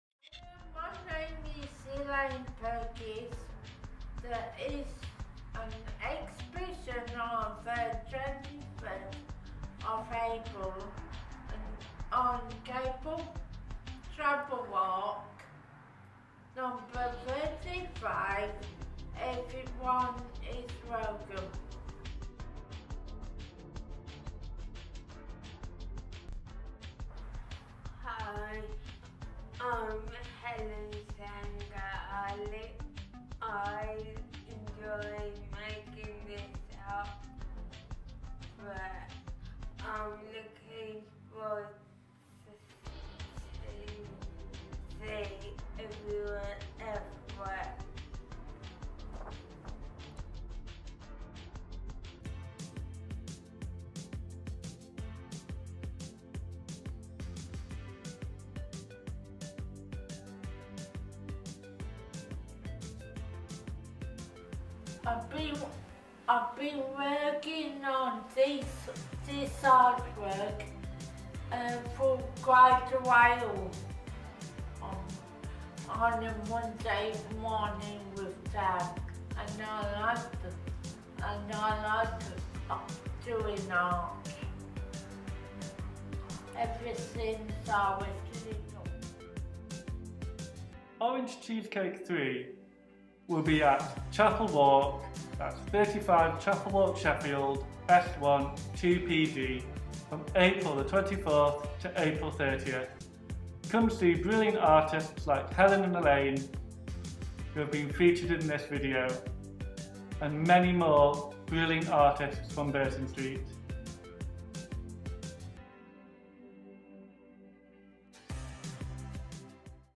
Orange Cheesecake 3 is coming!
Elaine and Helen introduce the new exhibition coming April 24th - April 30th.